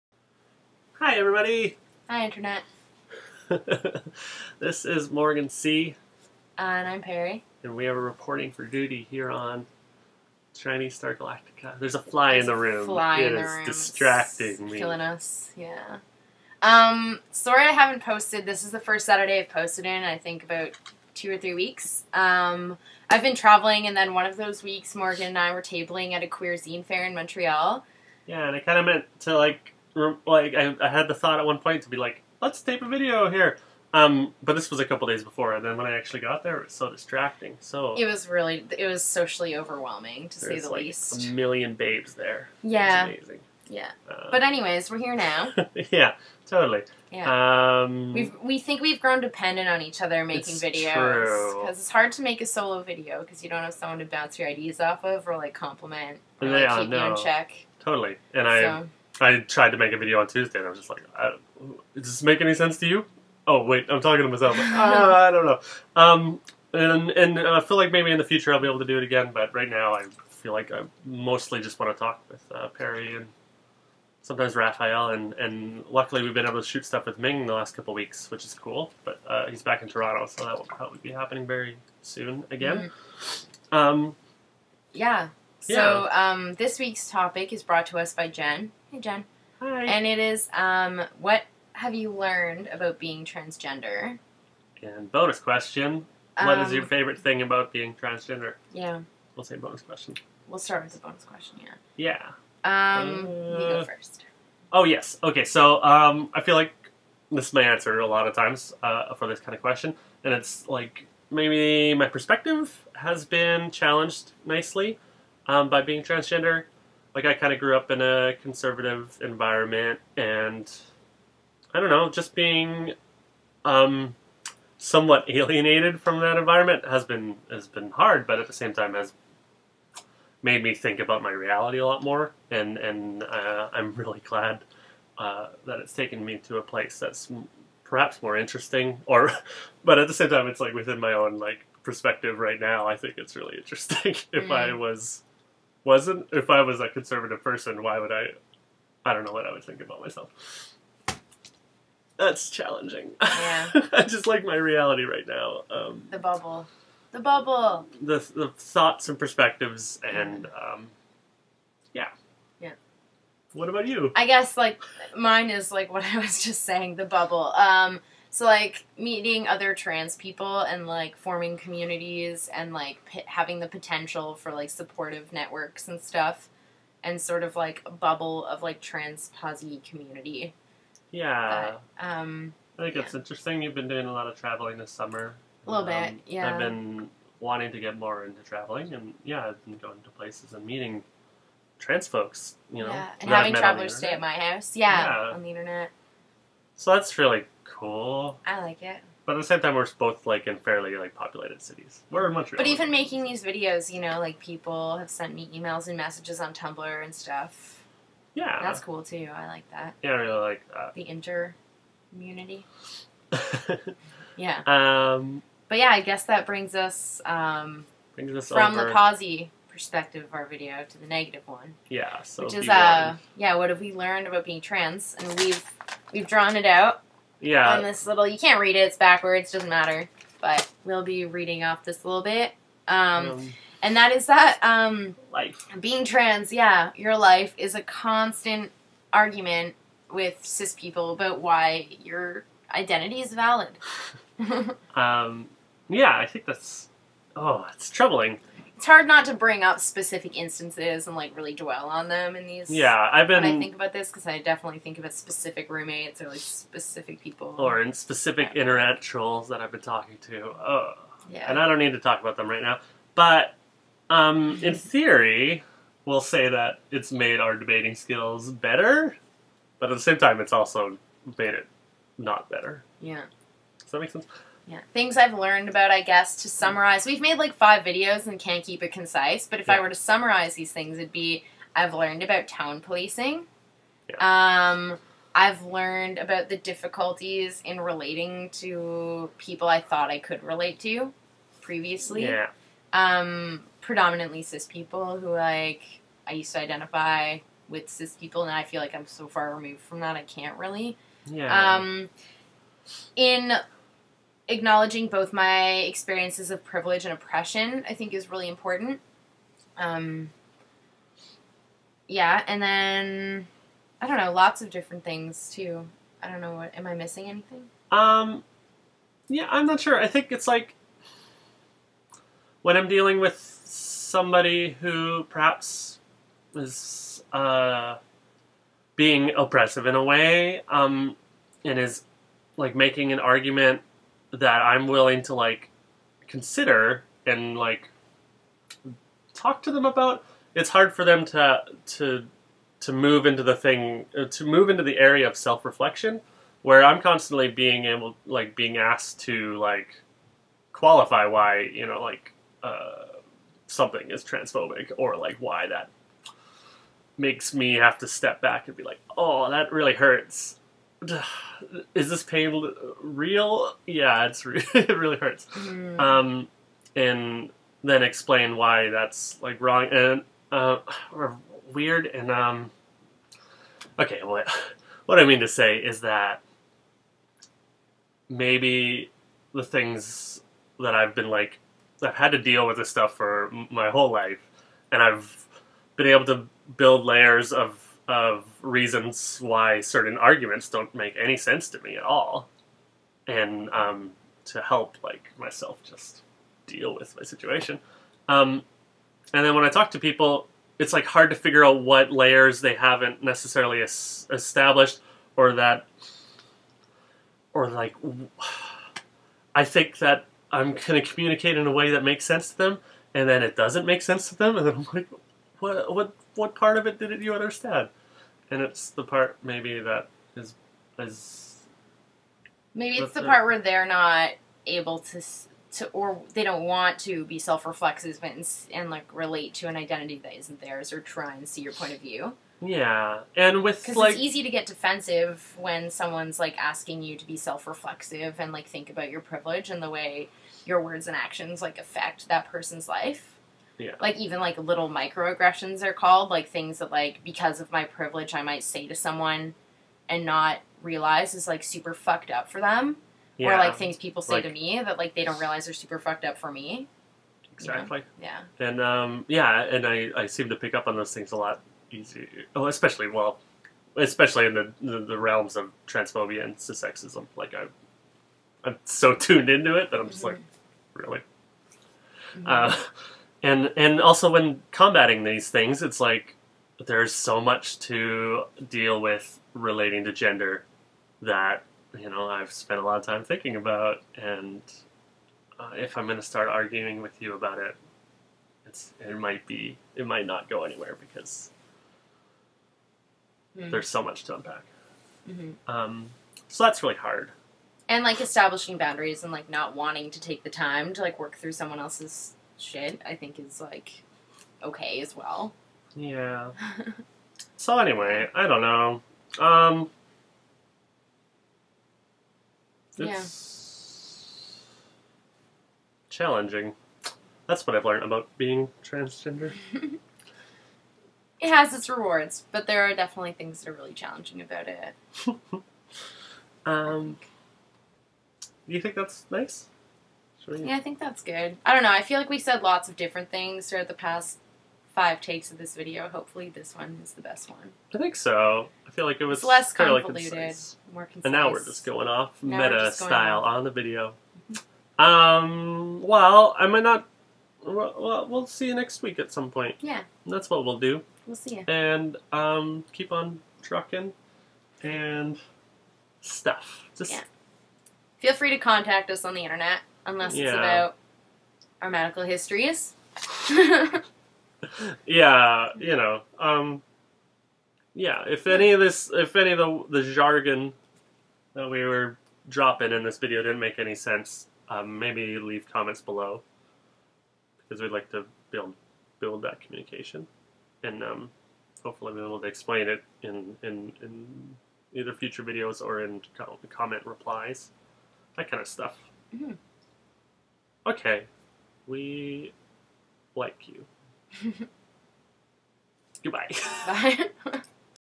TSG 208 - Master Debaters?
Perry and Morgan discuss trans community, communication, and conversing with cis people.. Neat!